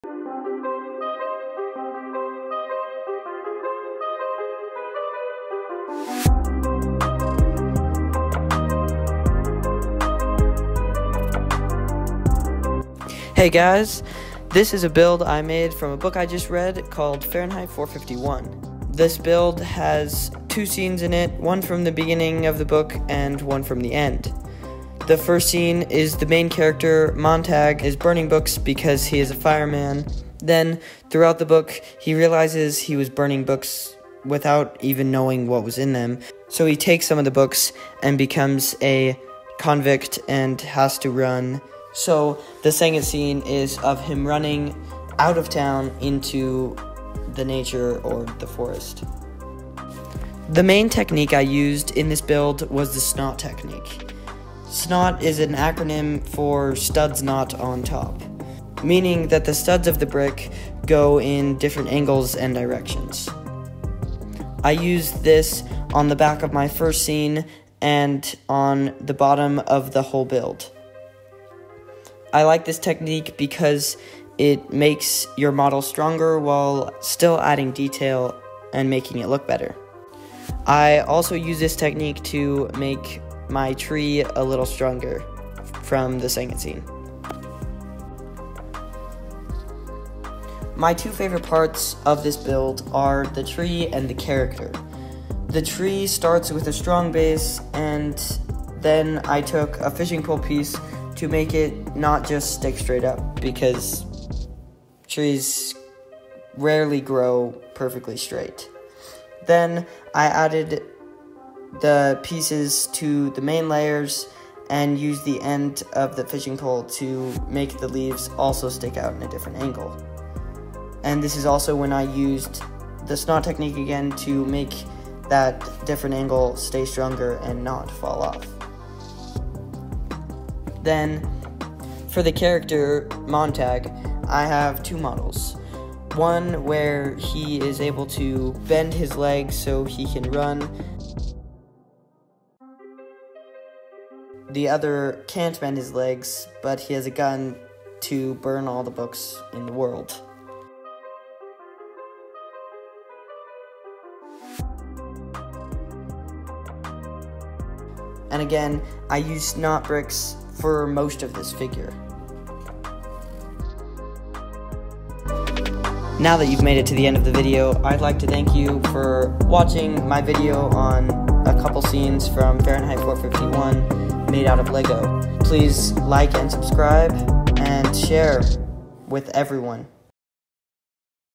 WILD LEGO scenes from Fahrenheit 451
This is a build I made that depicts two scenes from the classic book Fahrenheit 451! I use the SNOT technique in this build to add detail and make it stronger.